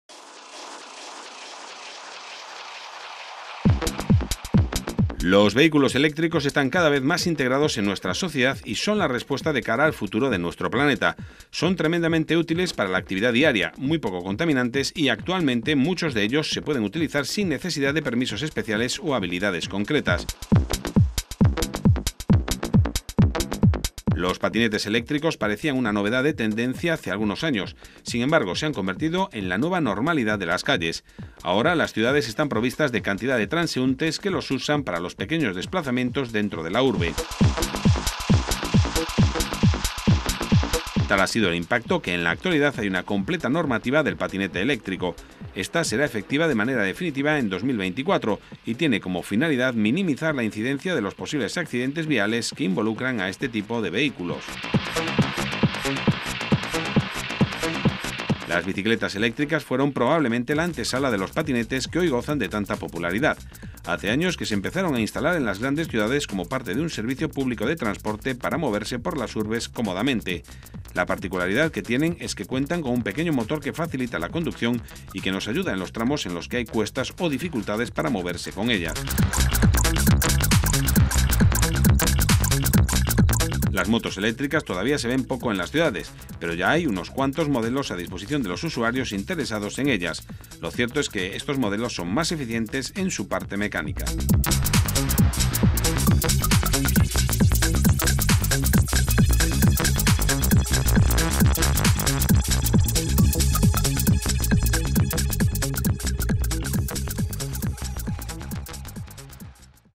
EL IMPARABLE AUGE DE LOS NUEVOS VEHÍCULOS ELÉCTRICOS | By CdRas
Los vehículos eléctricos están cada vez más integrados en nuestra sociedad y son la respuesta de cara al futuro de nuestro planeta. Son tremendamente útiles para la actividad diaria, muy poco contaminantes y, actualmente, muchos de ellos se pueden utilizar sin necesidad de permisos especiales o habilidades concretas.

Los patinetes eléctricos parecían una novedad de tendencia hace algunos años, sin embargo, se han convertido en la nueva normalidad de las calles. Ahora, las ciudades están provistas de cantidad de transeúntes que los usan para los pequeños desplazamientos dentro de la urbe.

Tal ha sido el impacto que, en la actualidad, hay una completa normativa del patinete eléctrico. Esta será efectiva de manera definitiva 2024 y tiene como finalidad minimizar la incidencia de los posibles accidentes viales que involucran a este tipo de vehículos.

Las bicicletas eléctricas fueron probablemente la antesala de los patinetes que hoy gozan de tanta popularidad. Hace años que se empezaron a instalar en las grandes ciudades como parte de un servicio público de transporte para moverse por las urbes cómodamente. La particularidad que tienen es que cuentan con un pequeño motor que facilita la conducción y que nos ayuda en los tramos en los que hay cuestas o dificultades para moverse con ellas.

Las motos eléctricas todavía se ven poco en las ciudades, pero ya hay unos cuentos modelos a disposición de los usuarios interesados en ellas. Lo cierto es que estos modelos son más eficientes en su parte mecánica.